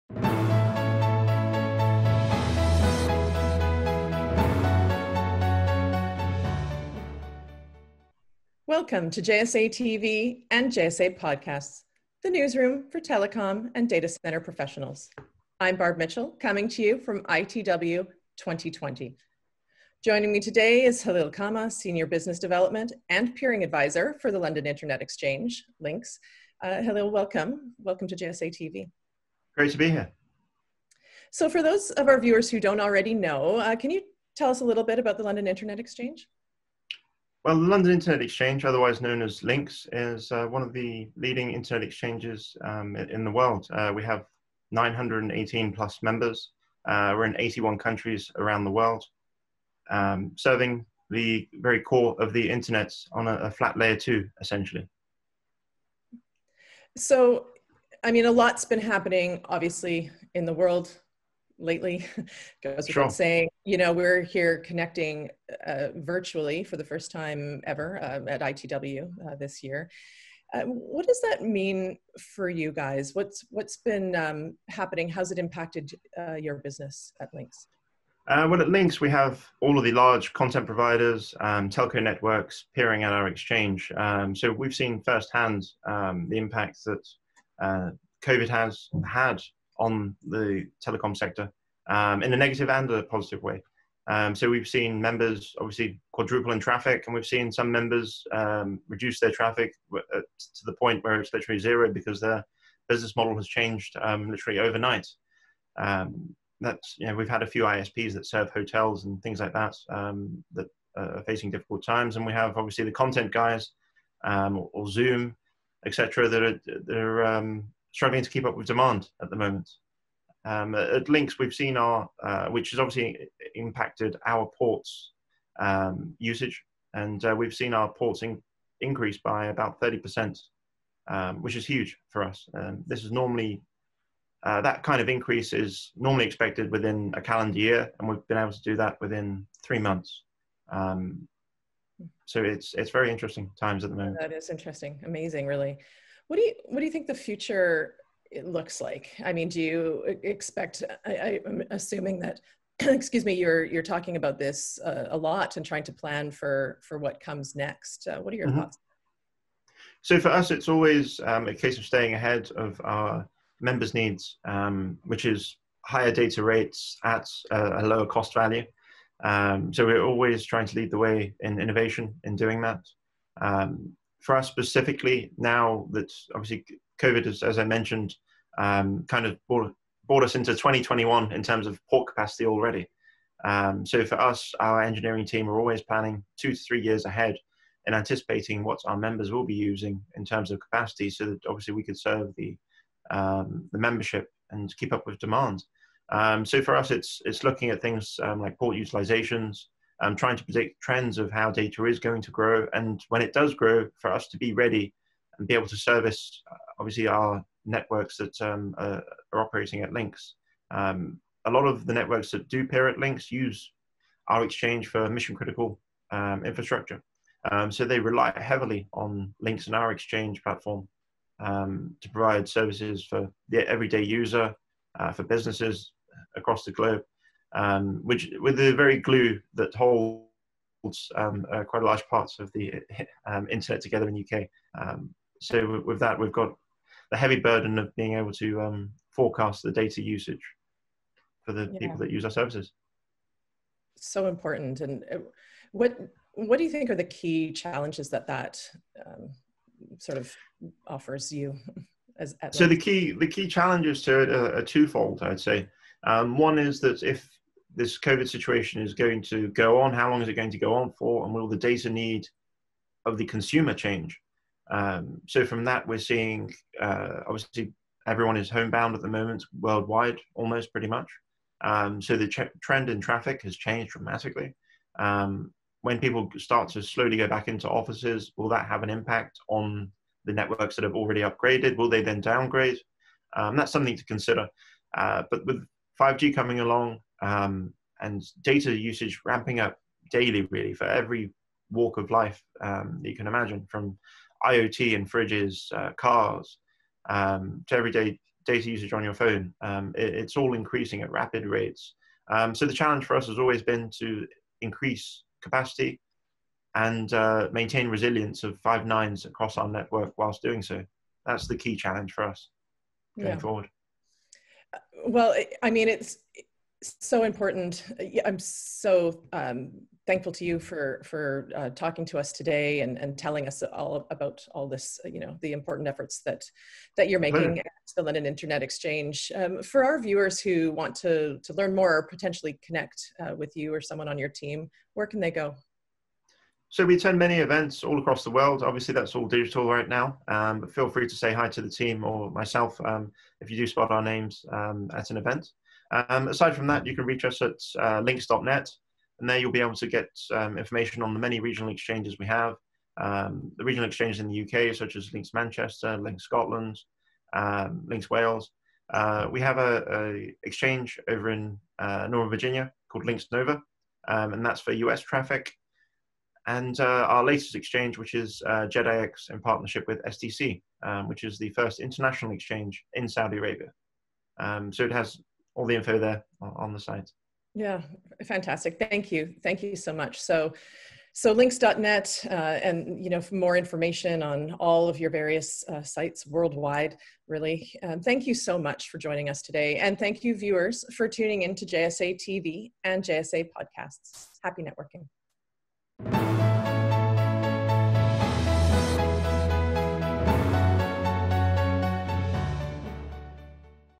ITW 2020: How the London Internet Exchange (LINX) is Preparing for the Future
Connecting virtually to ITW 2020, Halil Kama, Senior Business Development and Peering Advisor for the London Internet Exchange (LINX), provides important background to LINX, one of the leading Internet Exchanges in the world.  With service to over 918 members in 81 countries, and with all the large content providers and telco networks peering at its exchange, LINX has seen first hand the impact that COVID has had on the Telecom sector.   With the unprecedented growth and demand that LINX has experienced over the last few months, a 30% increase in port usage for example, it is more important than ever for the LINX engineering team to be planning for the years ahead to increase capacity and maintain resiliency.  LINX recognizes the importance of effectively predicting demand so that they can safeguard members’ mission critical infrastructure.

To find information on our regional exchanges

LINX Scotland
LINX Manchester
LINX Wales
LINX Nova
JEDIX